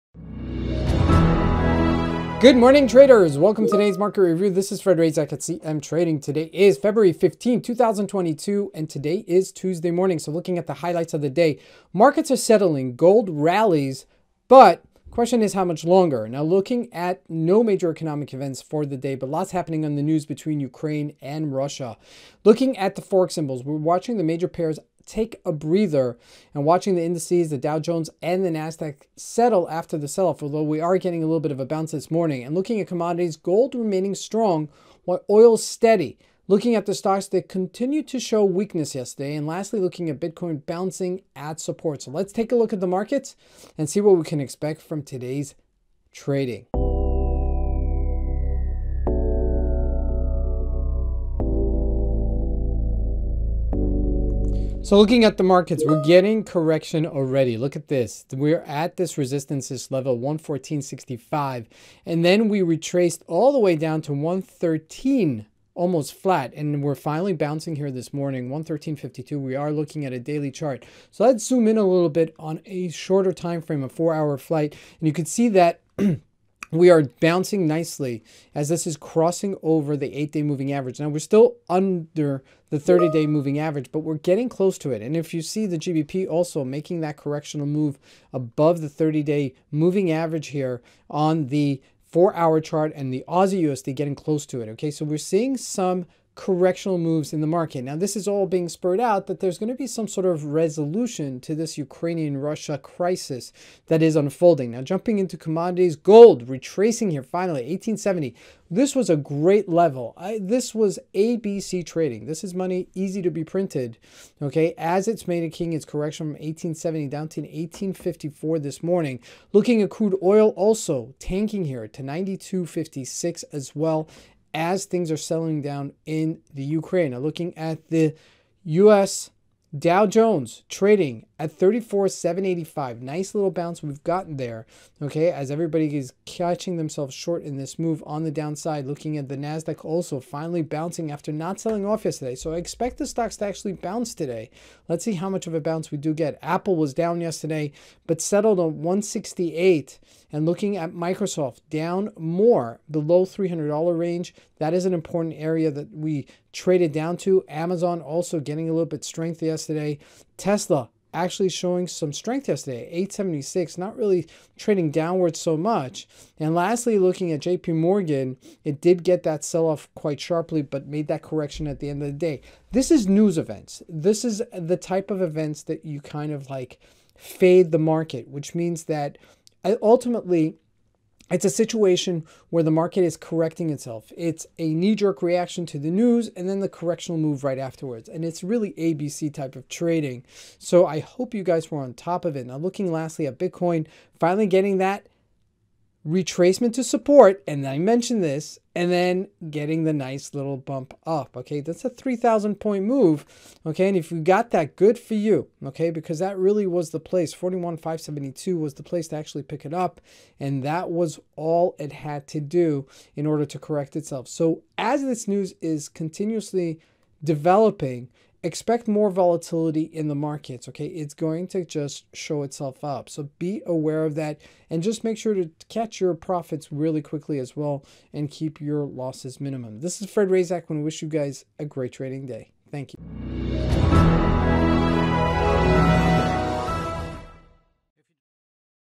CMTrading Daily Market Review February 15, 2022
HIGHLIGHTS OF THE DAY February 15, 2022

Markets settle, Gold rally’s higher, but…

[FOREX]   MAJOR PAIRS take a breather
[INDICES]   DOW JONES & NASDAQ settle after selloff
[COMMODITIES]   GOLD remaining strong, OIL steady
[STOCKS]   Continue to show weakness
[CRYPTO]   BITCOIN bounces at support